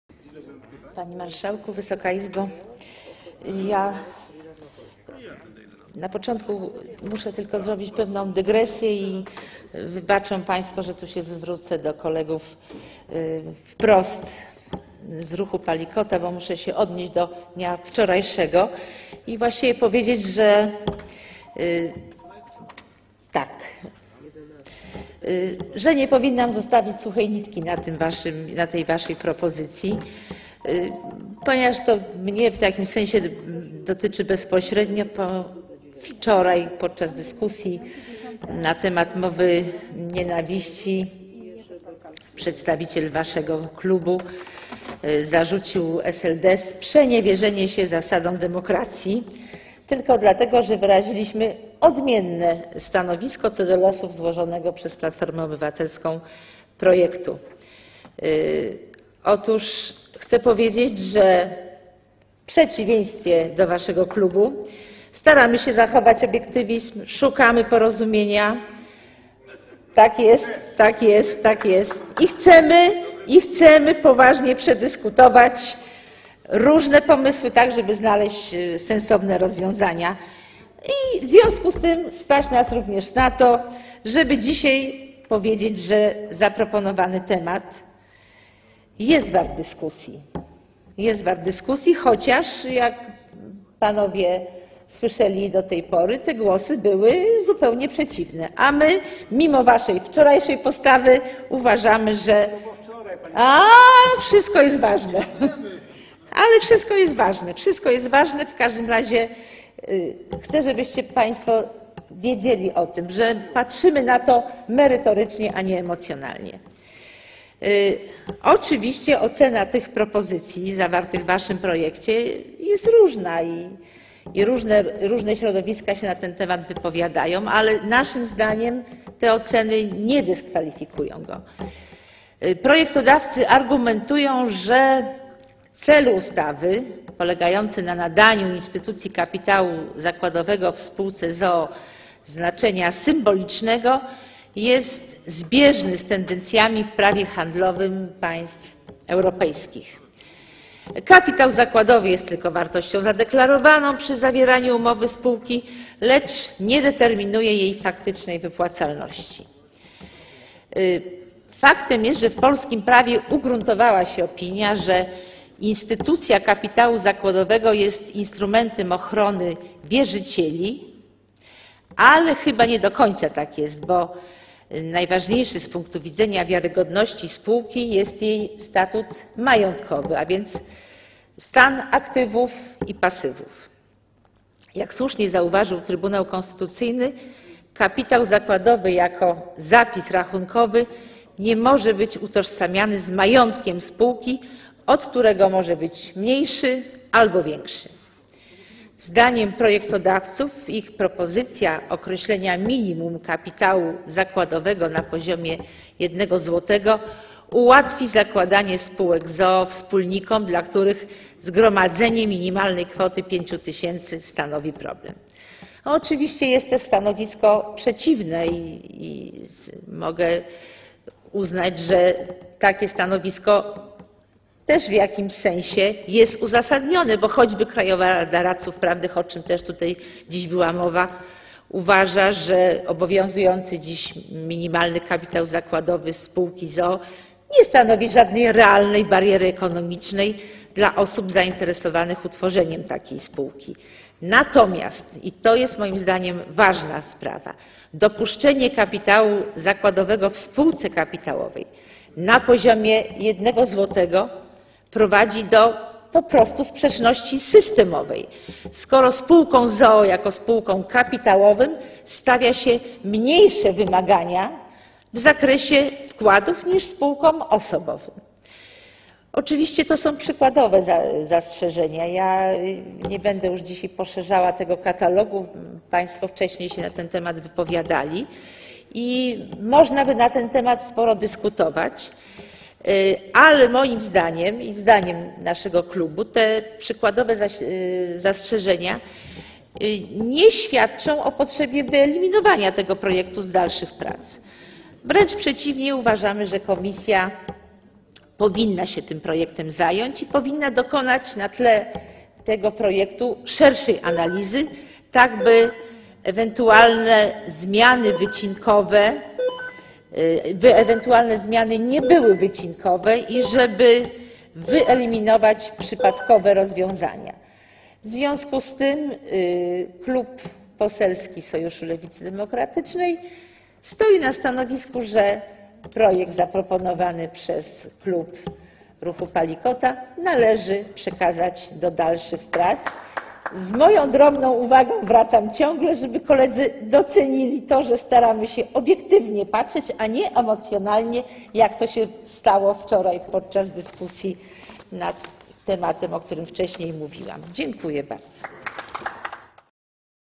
Małgorzata Sekuła Szmajdzińska - wystąpienie z 21 lutego 2013 r.
Popierasz? Udostępnij! Małgorzata Sekuła Szmajdzińska - wystąpienie z 21 lutego 2013 r.
Pierwsze czytanie poselskiego projektu ustawy o zmianie ustawy Kodeks spółek handlowych